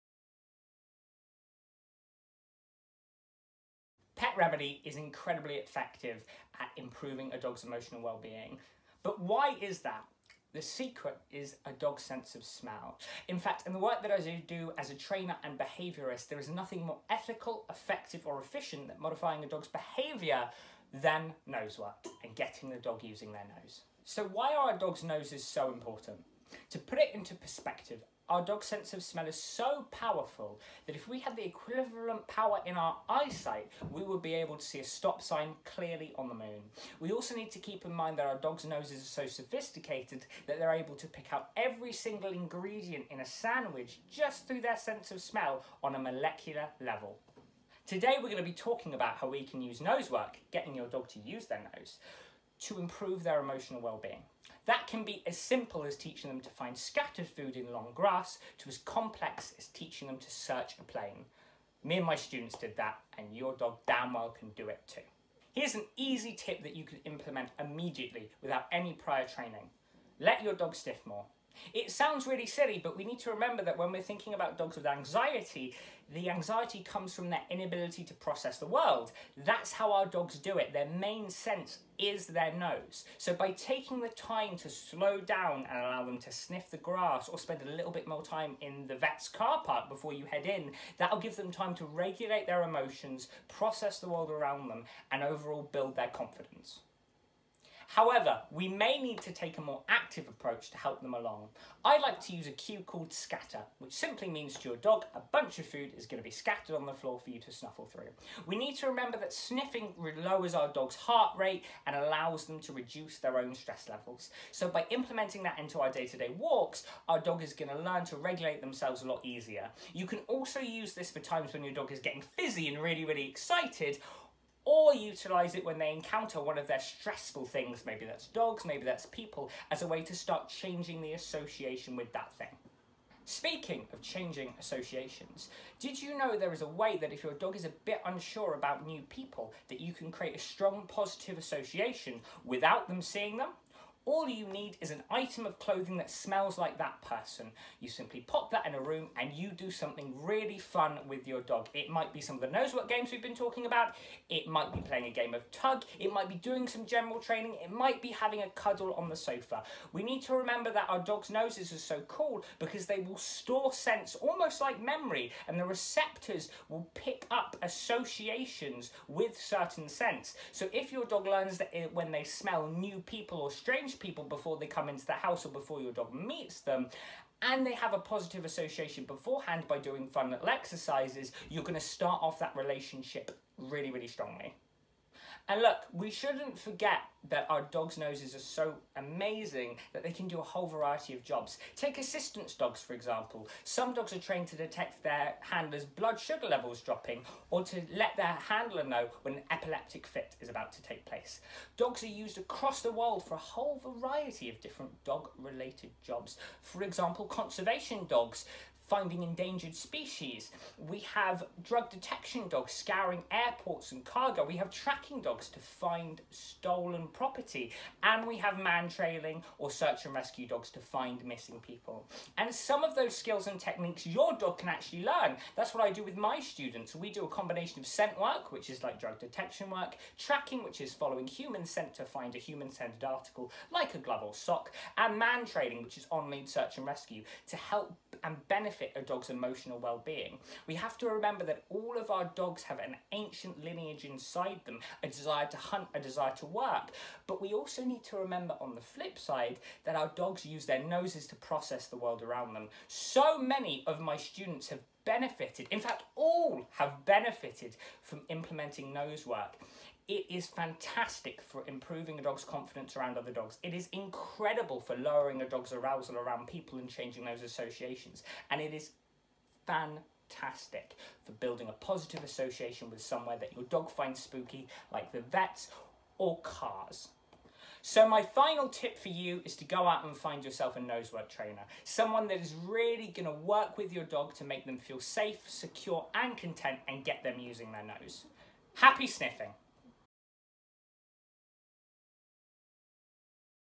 Jack Fenton talks about dog nose work
Jack Fenton talks about dog nose work. Understanding your dogs sense of smell and how to use this to give them better emotional wellbeing.
Part of the Pet Remedy Summer of Love 2023 talks